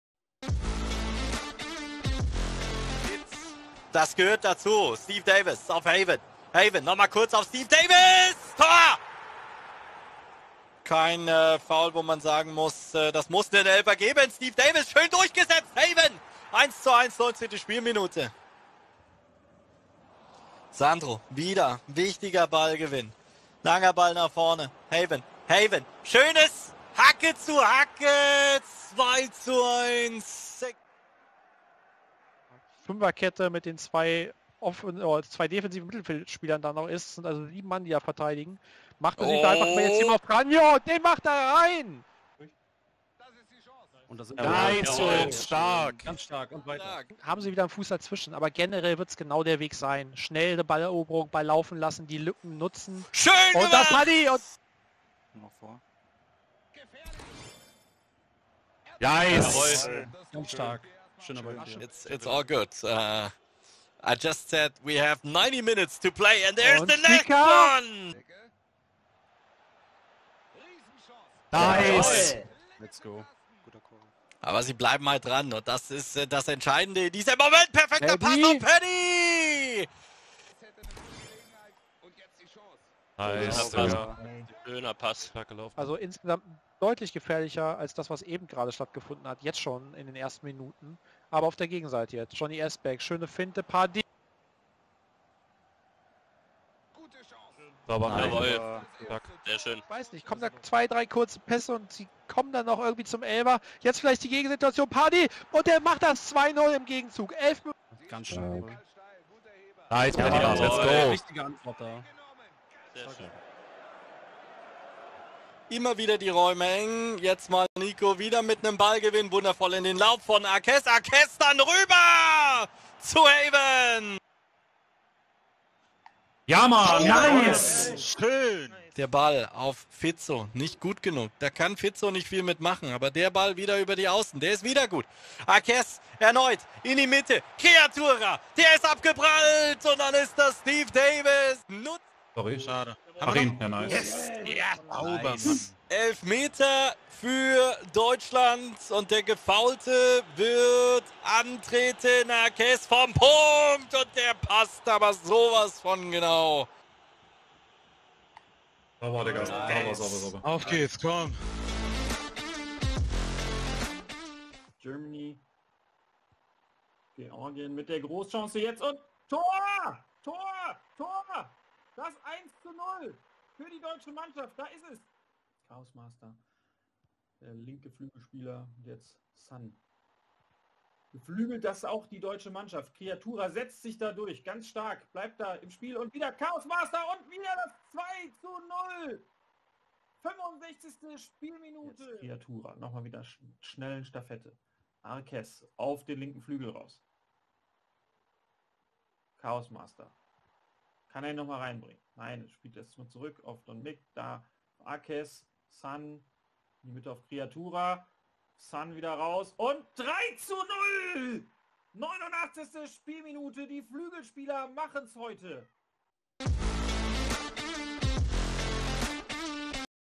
Alle Tore Deutschland mit Finale
Zum Schluss die Tore der Deutschen Nationalelf und das Finale noch einmal kurz zusammengefasst!